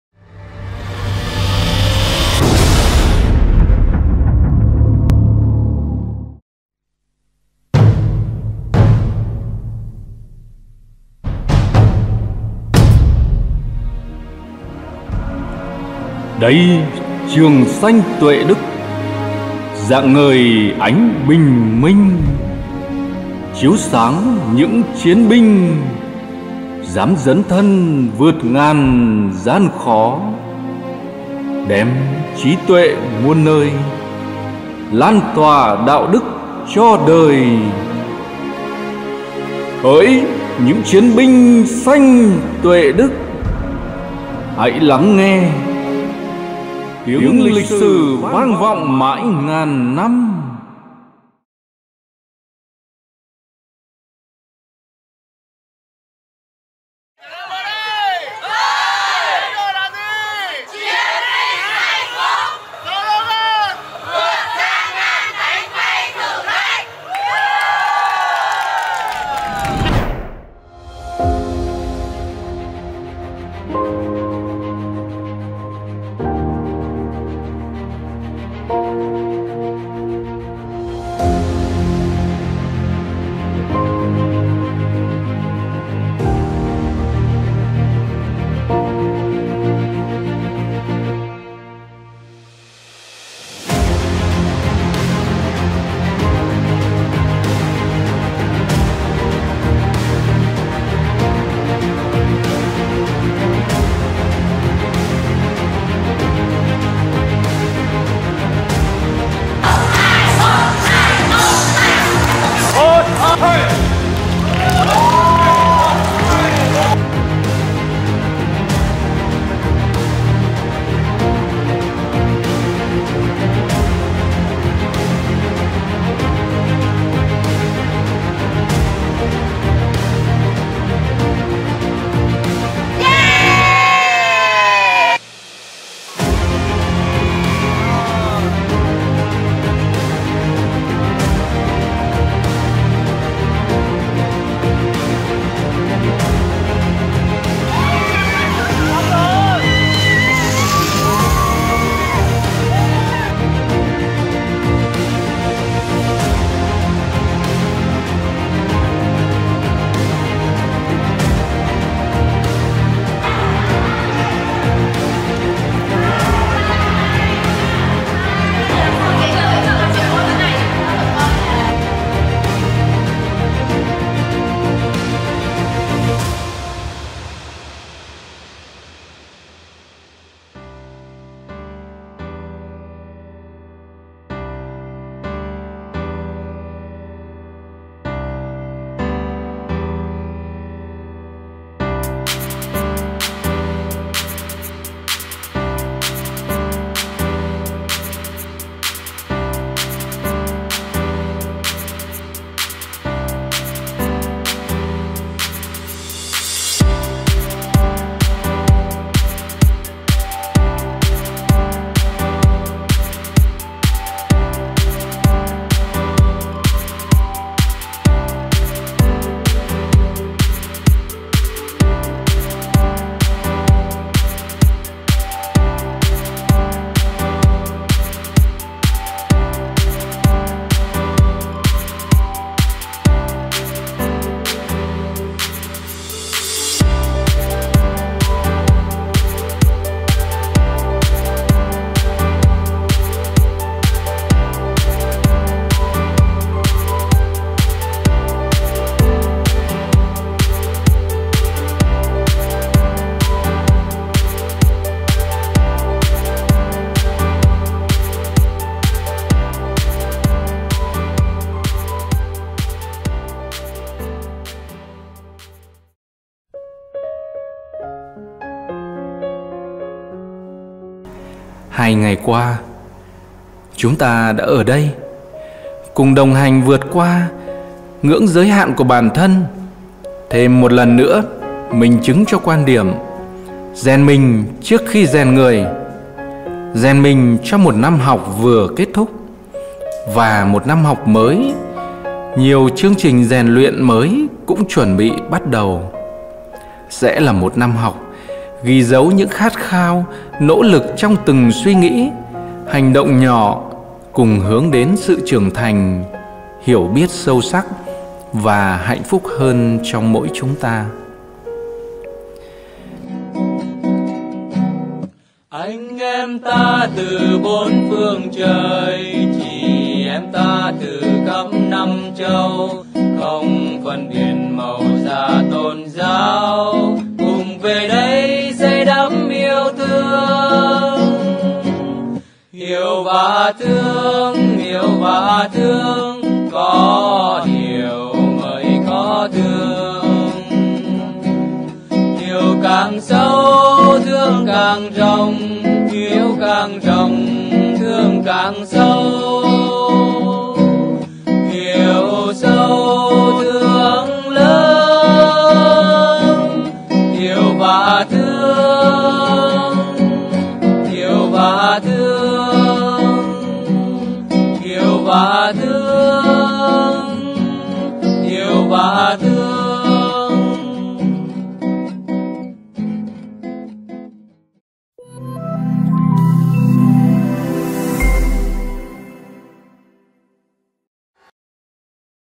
Rèn Luyện 2 Ngày 1 Đêm Của Thầy Cô Trường Xanh Tuệ Đức
Trường Tiểu học & Trung học cơ sở Xanh Tuệ Đức
Địa chỉ: Khu đô thị Thanh Hà, Hà Nội